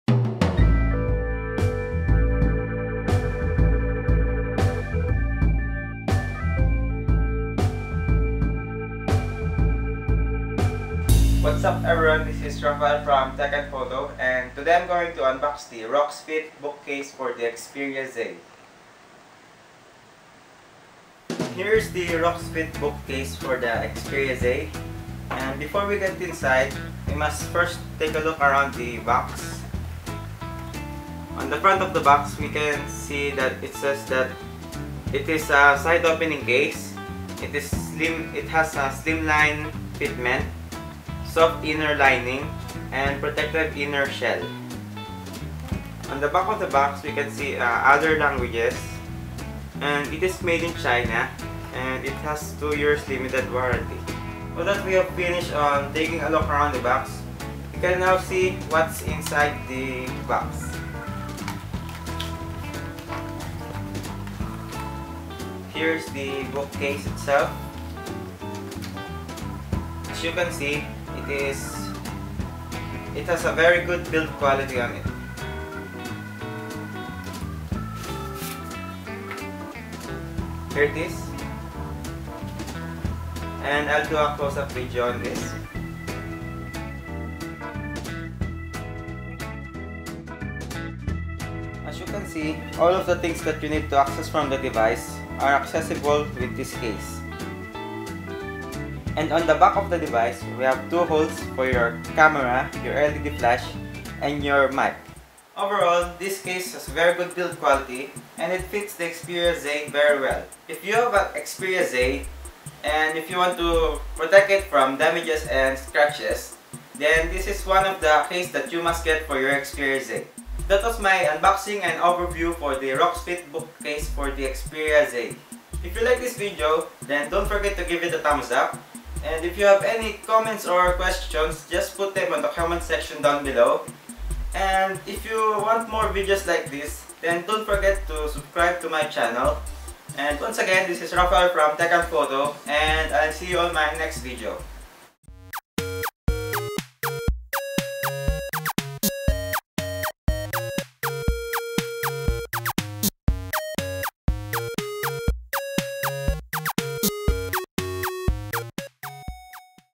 Roxfit Book Case for Sony Xperia Z Unboxing and Overview TechNPhoto
This is my unboxing and overview of the Roxfit Book case for the SONY Xperia Z.